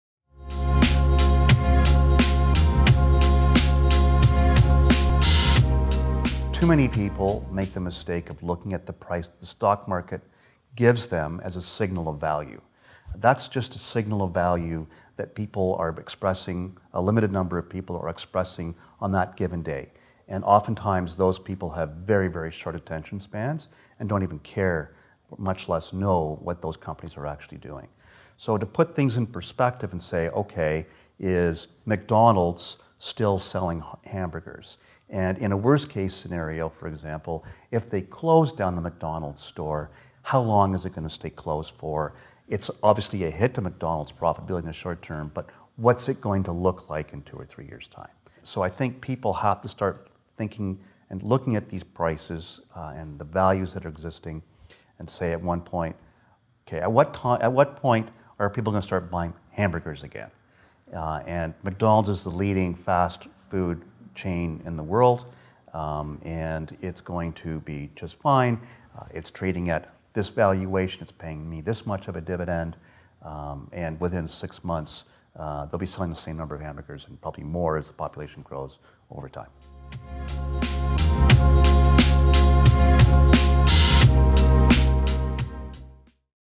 John O'Connell: Why Investors Mistake Price for Value
One of the biggest mistakes investors make is misjudge the price of a stock and the value of the underlying business, according to John O'Connell, Chairman and CEO of Davis Rea Investment Counsel. He explains why in this conversation with Mark Bunting.